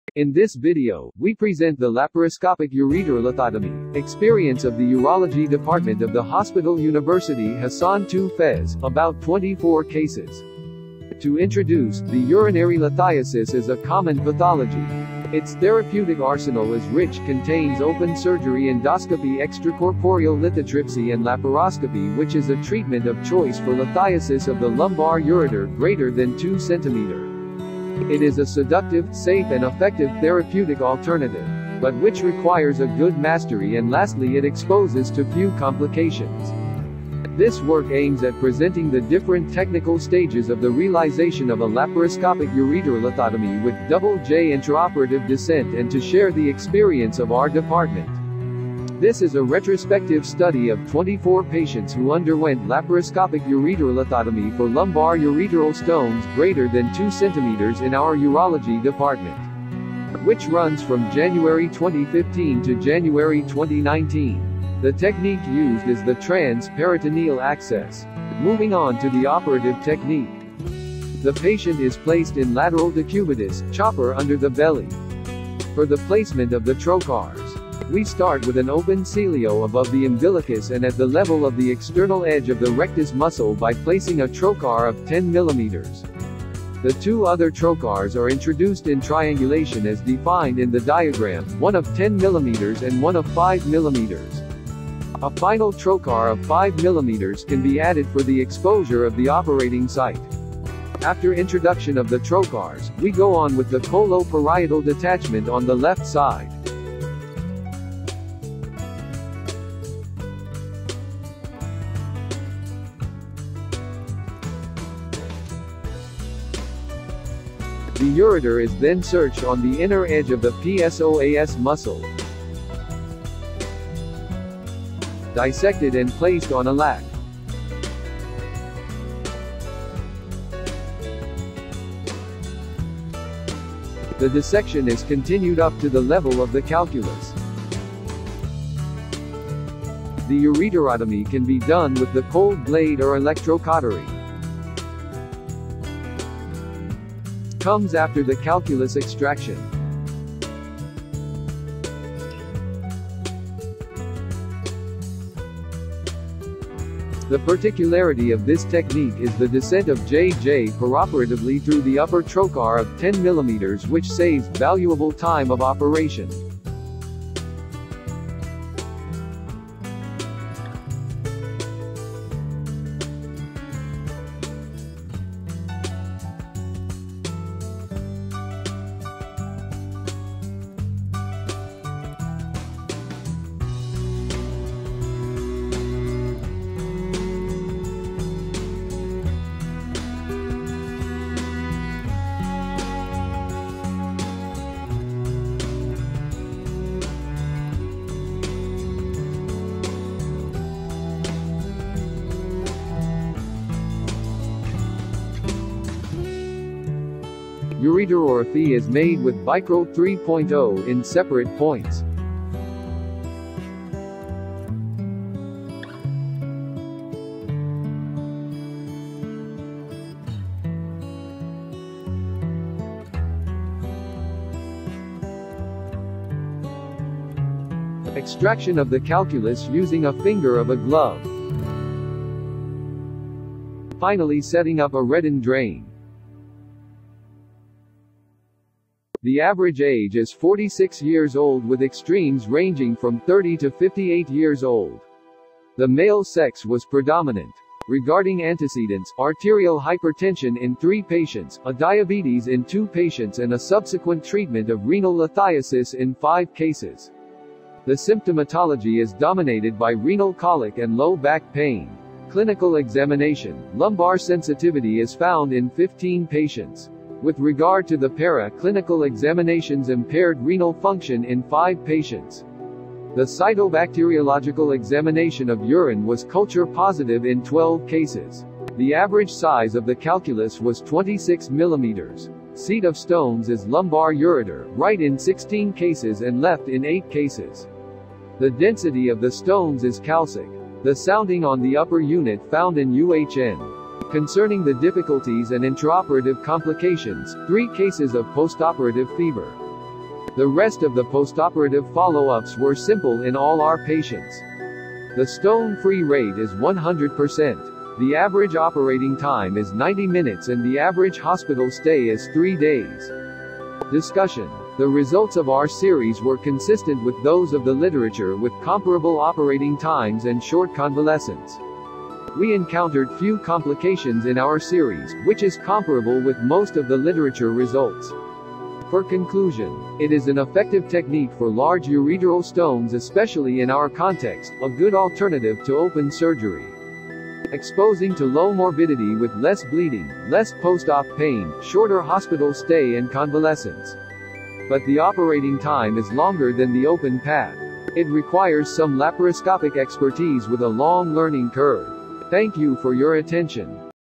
Laparoscopic ureterolithotomy for lumbar stone
C'est une vidéo didactique qui montre l’intérêt de la chirurgie laparoscopique pour le traitement des calculs de l’uretère lombaire avec la présentation de notre série réalise dans le CHU de Fes (Maroc) par le Professeur AHSAINI Mustapha.
Bonne lecture.